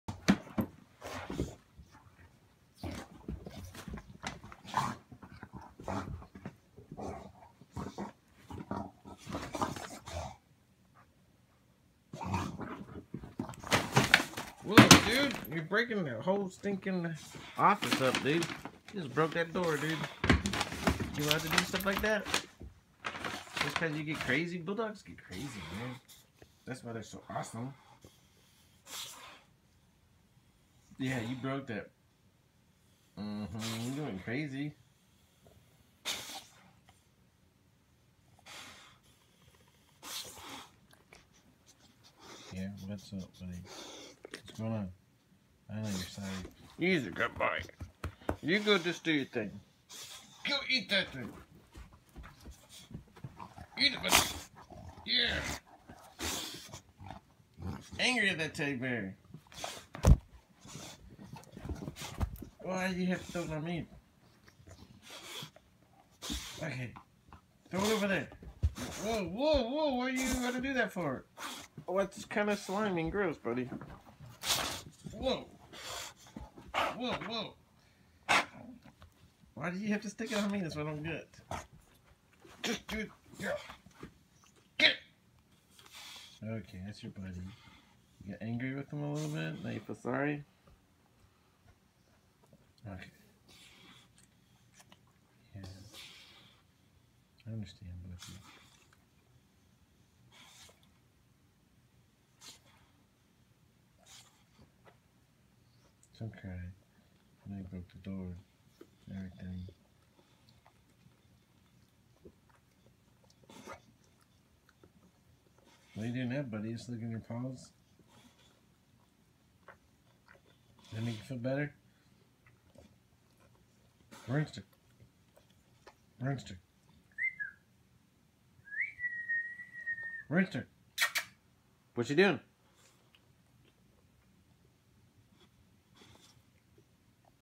Rooney getting crazy
Rooney the Awesome bulldog going crazy.    Every day bulldog stuff is awesome. check out more Rooney Videos